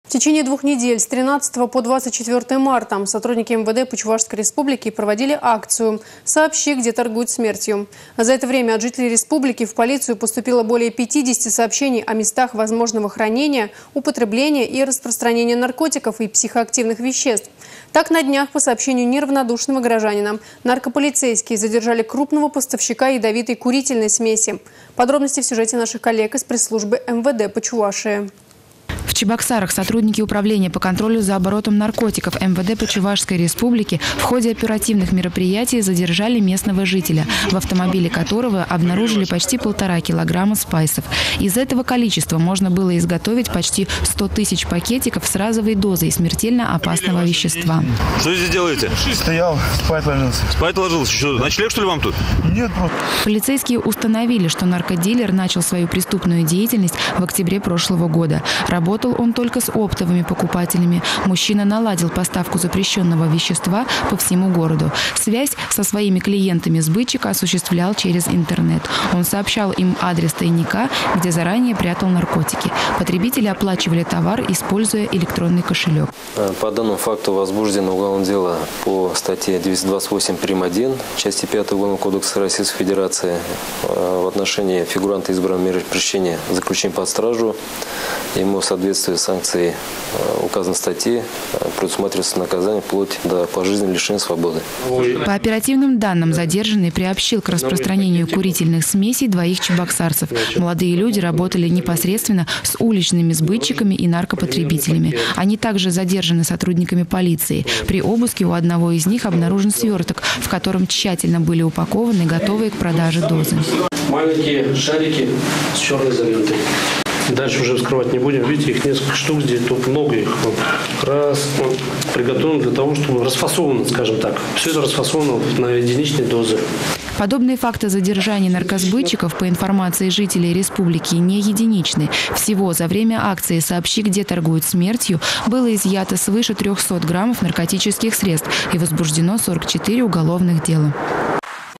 В Чебоксарах полицейские пресекли незаконный сбыт наркотиков в особо крупном размере
В течение двух недель, с 13 по 24 марта, сотрудники МВД по Чувашской Республике проводили акцию «Сообщи, где торгуют смертью». За это время от жителей республики в полицию поступило более пятидесяти сообщений о местах возможного хранения, употребления и распространения наркотиков и психоактивных веществ. Так, на днях по сообщению неравнодушного горожанина наркополицейские задержали крупного поставщика ядовитой курительной смеси. Подробности в  сюжете наших коллег из пресс-службы МВД по Чувашии.